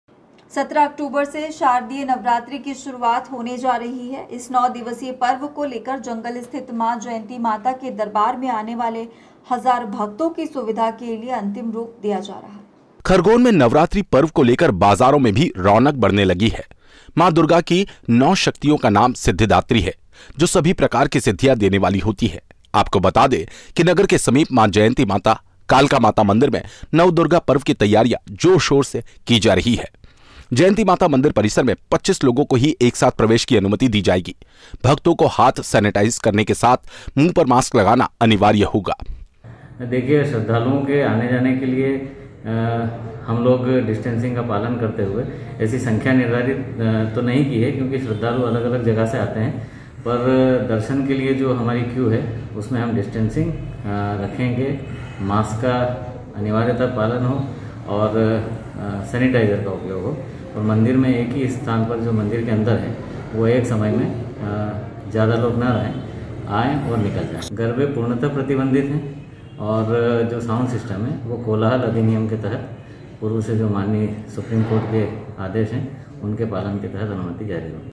17 अक्टूम्बर से नवरात्रि पर्व का होगा आगाज, माता मंदिरों में साज सज्जा की तैयारिया हुई शुरू !
BREAKING NEWS

ACN भारत  News Channel:

ACN भारत |  भारत का सर्वश्रेष्ठ हिंदी न्‍यूज चैनल है । ACN भारत न्‍यूज चैनल राजनीति, मनोरंजन,